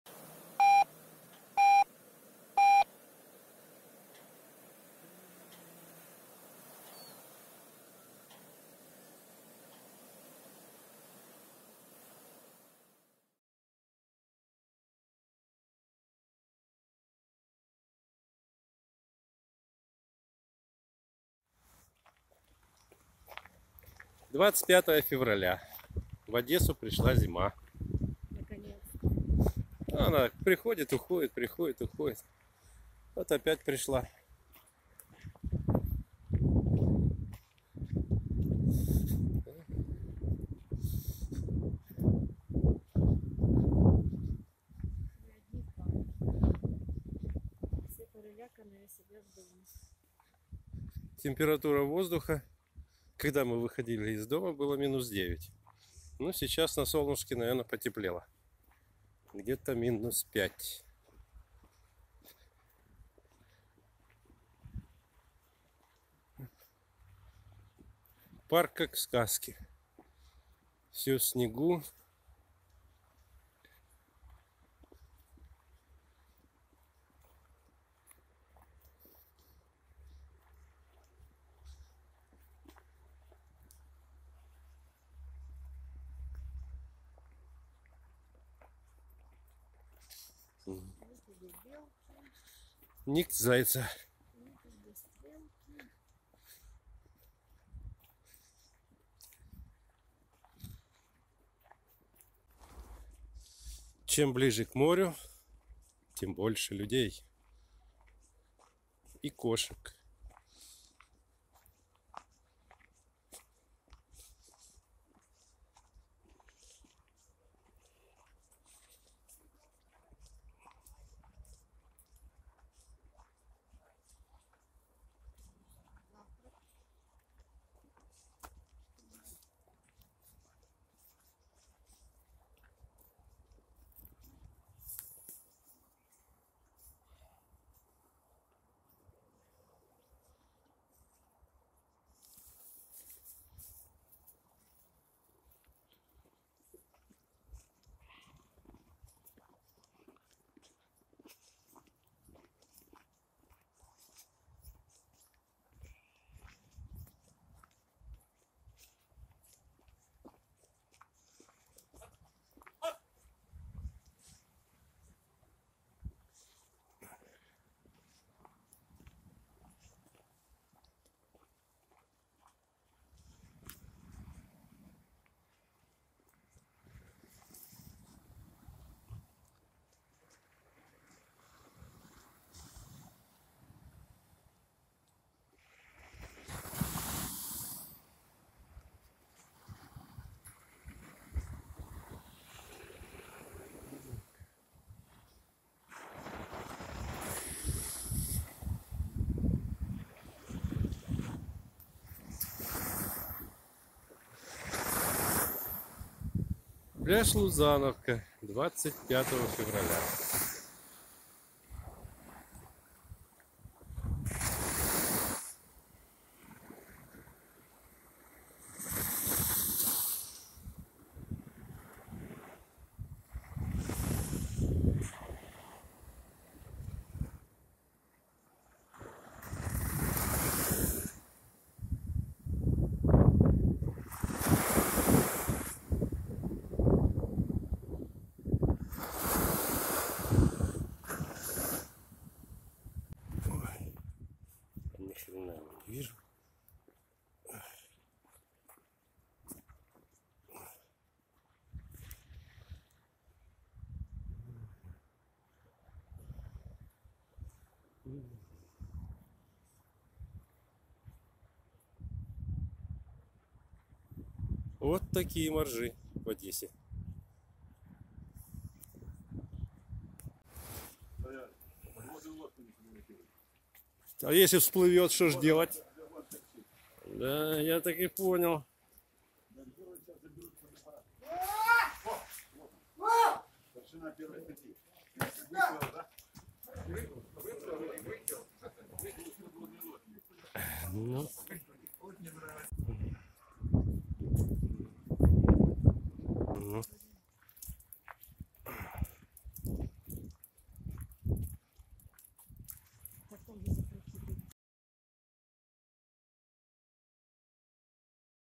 Зима в Одессе.
Зима в Одессе. 23 Февраля в Одессу вернулась зима, а 25 и морозец.